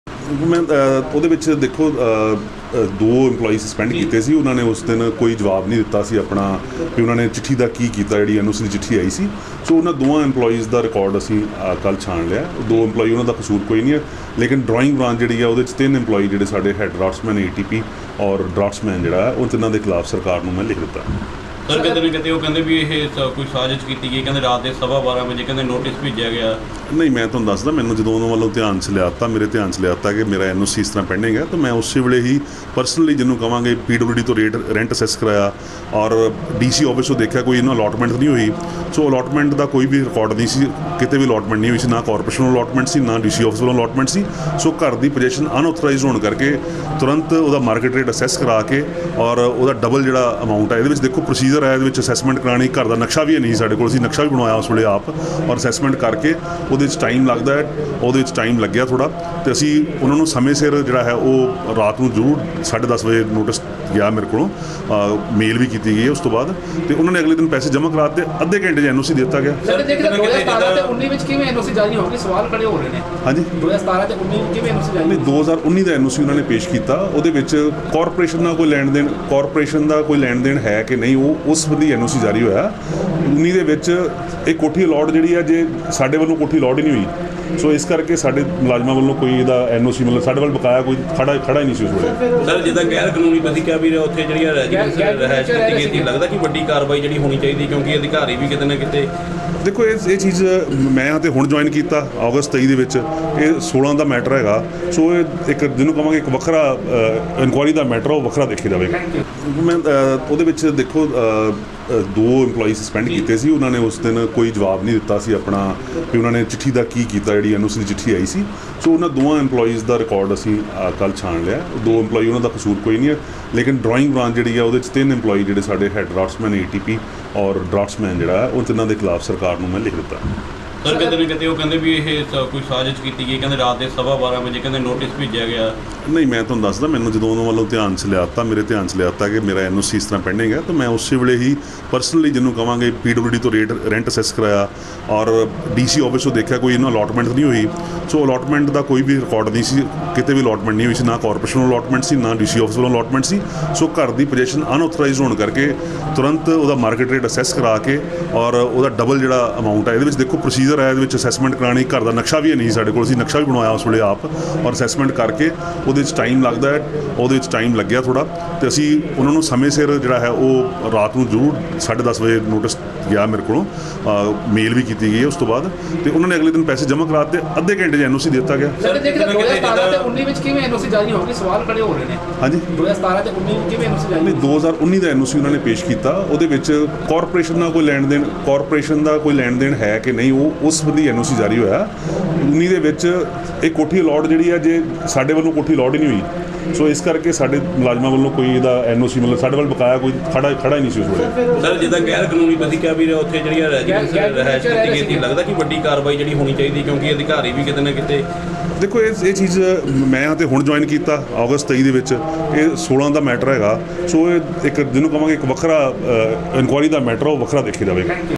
Ravneet Singh Bittu ਦੀ ਕੋਠੀ ਦੇ ਮਾਮਲੇ ਵਿਚ Nagar Nigam Commissioner ਦੇ ਵੱਡੇ ਖ਼ੁਲਾਸੇ
ਕਿਹਾ, ਗ਼ੈਰ ਕਾਨੂੰਨੀ ਢੰਗ ਨਾਲ ਰਹਿ ਰਹੇ ਸਨ Bittu